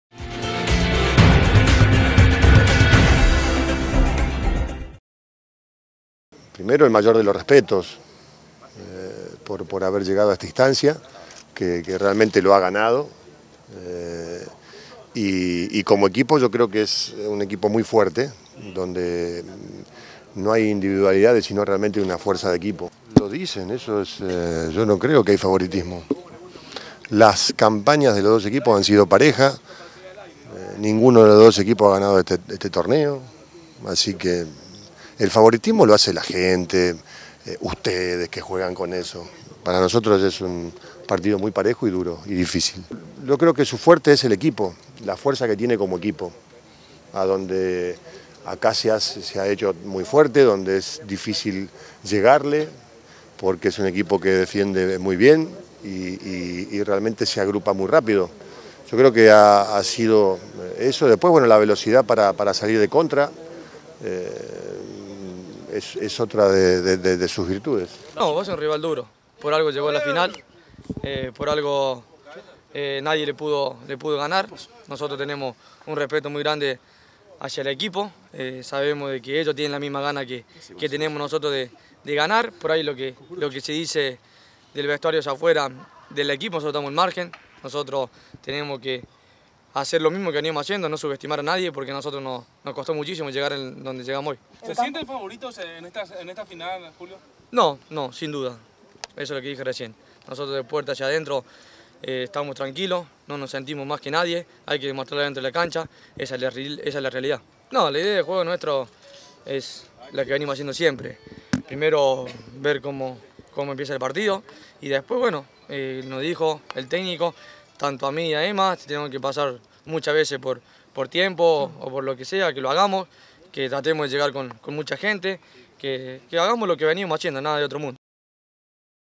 San Lorenzo respeta a Nacional - Previa final Copa Libertadores 2014
Mucho respeto hacia el rival es lo que tienen los integrantes del plantel de San Lorenzo de Almagro, según declararon posterior al reconocimiento del Defensores del Chaco que realizaron en la víspera a la primera final de la Copa Libertadores de América. Edgardo Bauza y Julio Buffarini dialogaron en conferencia tras la sesión futbolística.